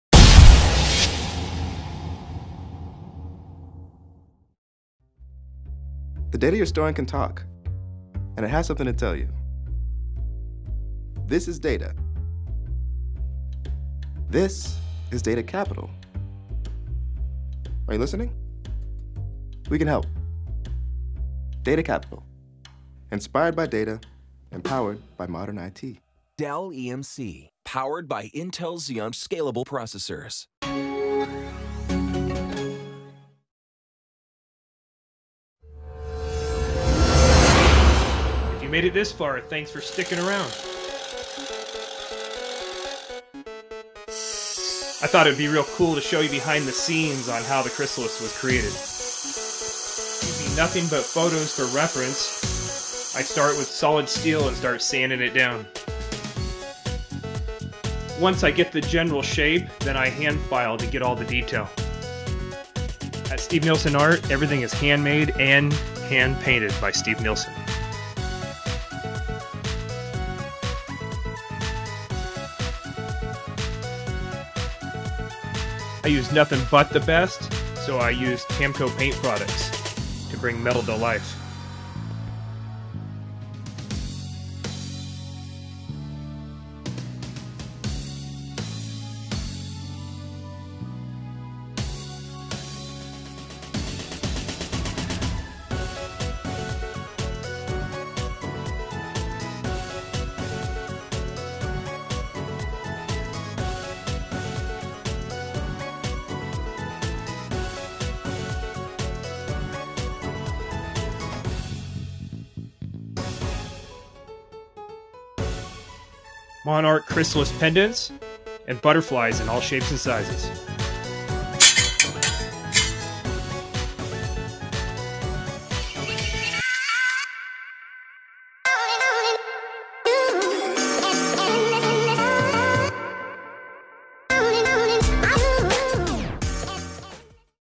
Monarch butterfly chrysalis for Dell EMC commercial.
The Dell video was produced for Dell EMC by The Distillery using a custom commissioned two inch monarch chrysalis made by artist Steve Nielsen.

 The behind the scenes footage was added at the end of the commercial, showing how the chrysalis was created. This piece was hand shaped from solid steel then painted using all Tamco Paint company products.

 Steve Nielsen Art was given permission to use the Dell video by The Distillery to share on all social platforms.